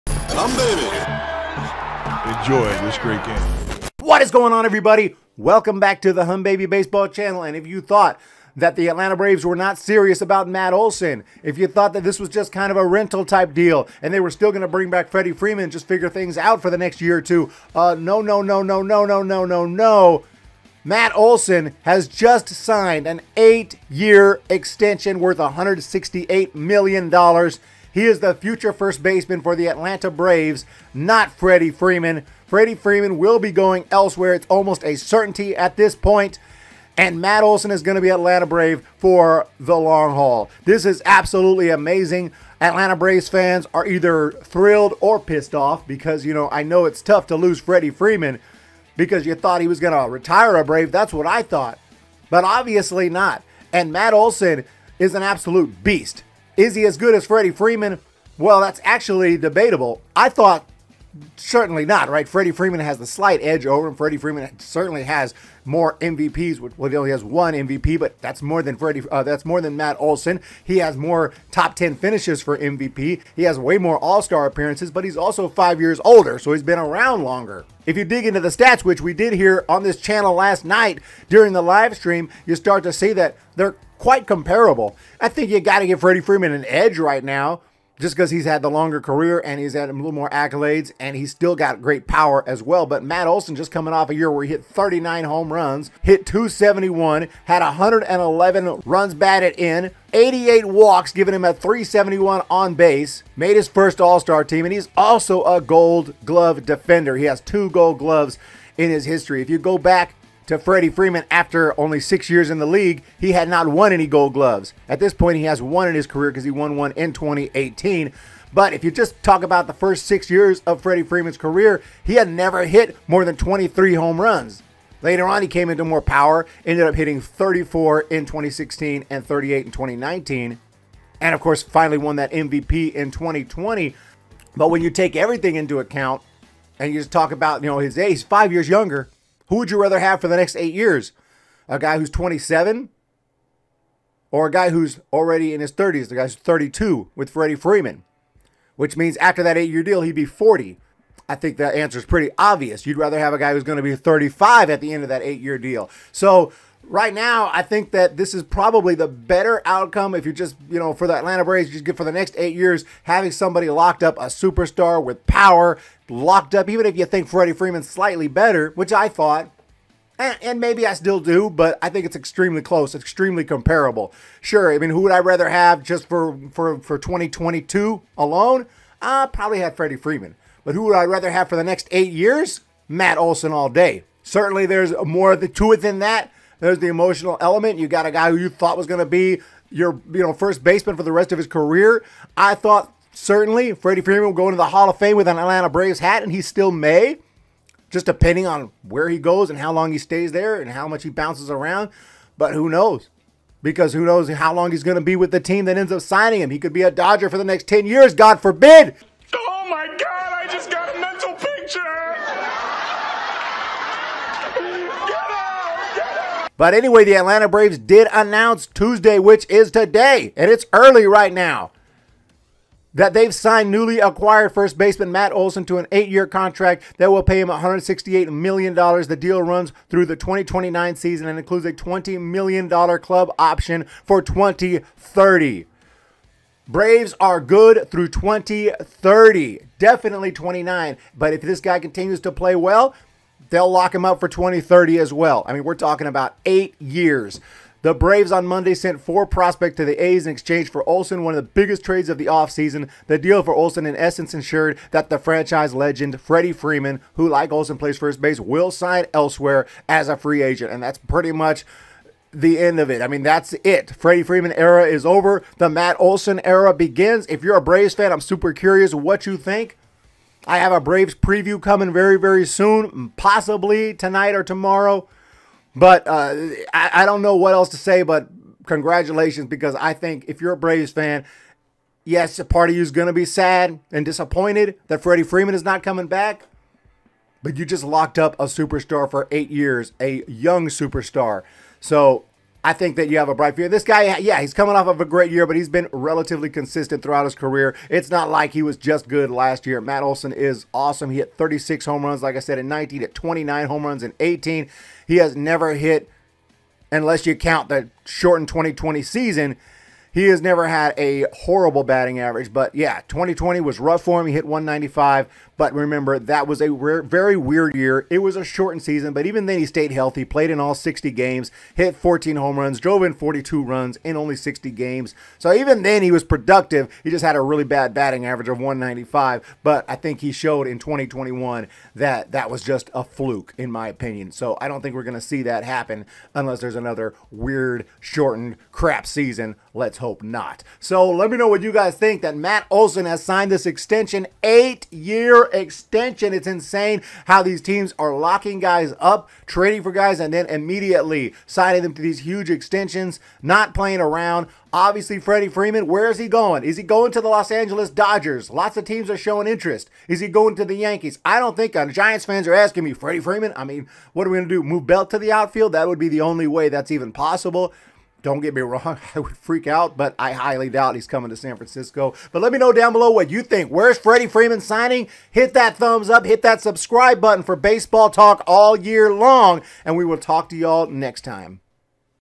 BRAVES EXTEND MATT OLSON!! 8 YEARS $168 Million.. End Of The Freddie Freeman Era In Atlanta?
The Atlanta Braves on Tuesday announced that they've signed newly acquired first baseman Matt Olson to an eight-year contract extension that will pay him $168 million. The deal will run through the 2029 season and includes a $20 million club option for 2030.

The Braves on Monday sent four prospects to the A's in exchange for Olson in one of the biggest trades of the offseason. The deal for Olson in essence ensured that franchise legend Freddie Freeman, who like Olson plays first base, will sign elsewhere as a free agent.